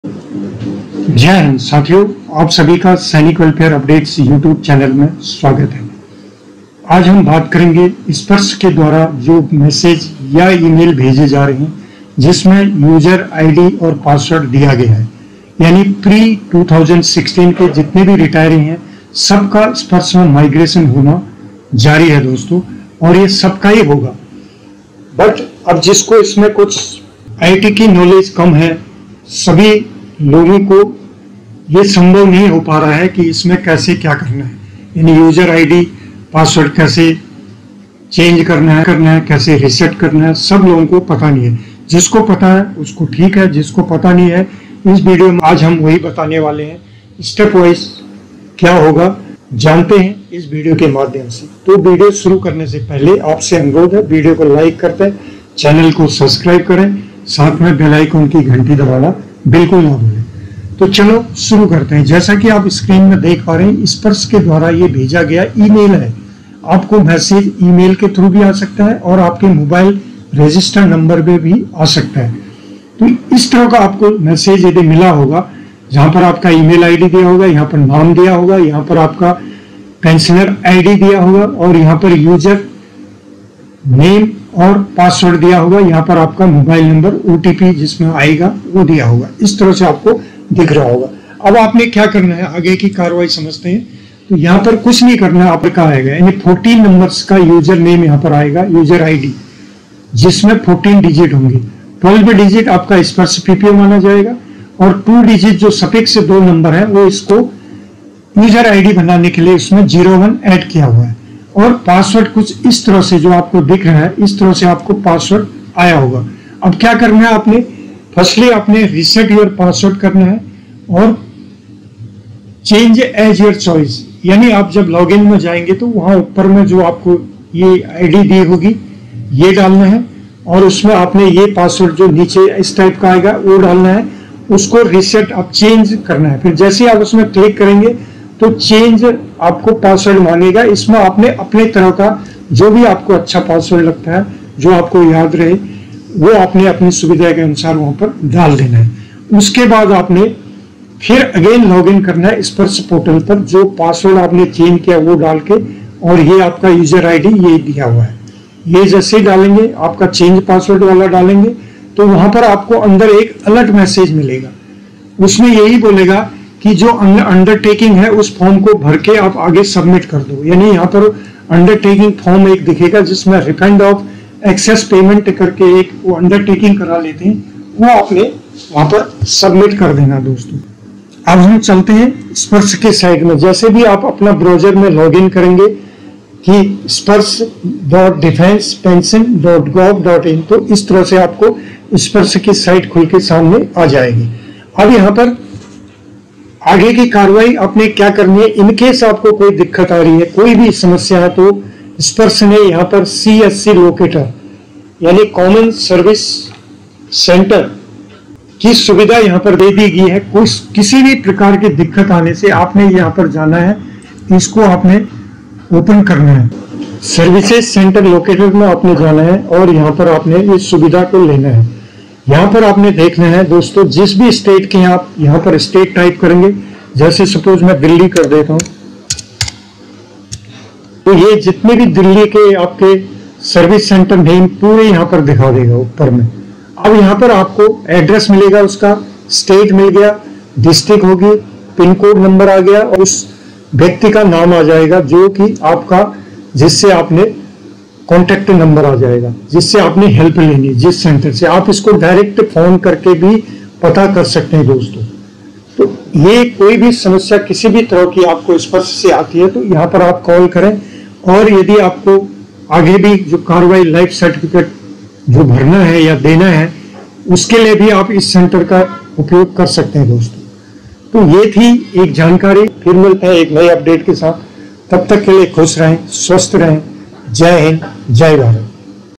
SPARSH Update- स्पर्श ने पेंशनर्स को दे दी बड़ी खुसखबरी | अब पेंशन लेना होगा आसान
*इस वीडियो मै आपको स्पर्श द्वारा भेजे गए यूजर नेम और पासवर्ड मिलने के बाद क्या करना है |
डेमो के साथ बताया गया है |
क्या स्पर्श से आपको भी इस तरह का मैसेज मिला है ?
YOUR USER’S NAME -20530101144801 (User ID)
14 digits (12-digit SPARSH PPO,
02 Digit prefix for user ID)
PASSWORD - ygC1bKVBt0
क्या करें?
1. Reset your Password- (Change-as your choice)
2. Login your SPARSH portal.
3. Alert Message.
4. Undertaking of Refund of excess payment
 Channel Aim is educate the people and serve for nation
"Jia Hind Jai Bharat"